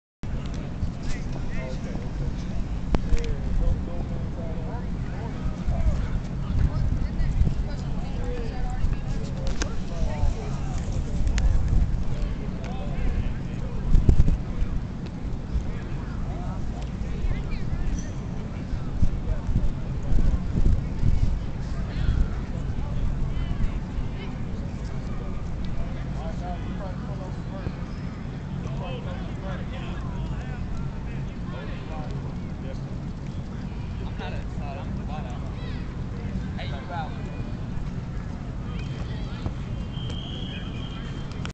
Landen Leiser @ 2013 Raleigh O-D Offense Defense Football camp
14-year old Quarterback Landen Leiser wearing the yellow helmet.  Landen stands 6' & weighs 140 pounds during the summer session.